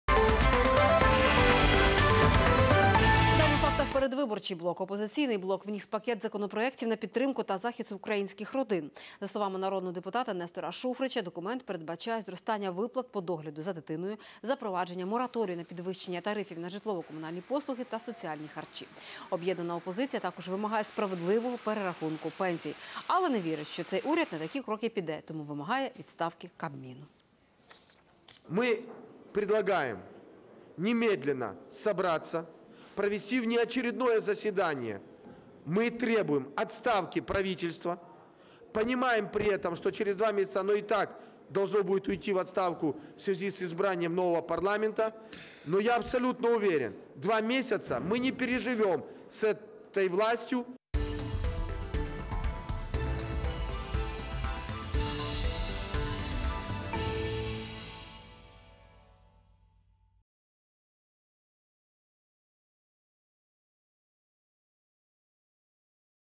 Шуфрич требует отставки Кабинета Министр, который скоро и так самораспустится
Оппозиционный блок внес пакет законопроектов в поддержку и защиту украинских семей, но не верит, что это правительство на такие шаги пойдет, поэтому требует отставки Кабмина.

Опозиційний блок вніс пакет законопроектів на підтримку та захист українських родин, але не вірить, що цей уряд на такі кроки піде, тому вимагає відставки Кабміну.